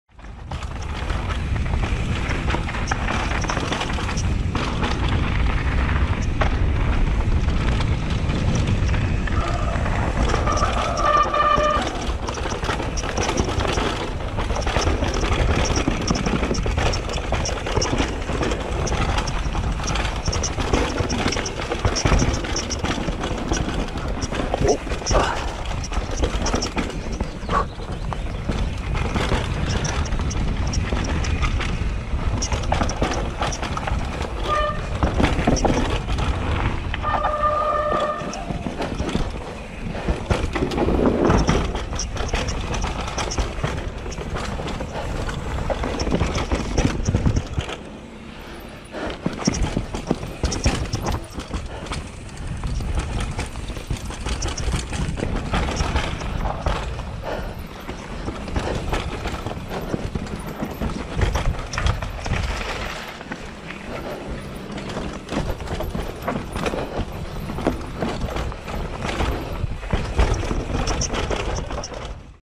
Bike check, roots and rocks.
A good reason to tighten everything with thread locker. If anything is loose you will hear it here.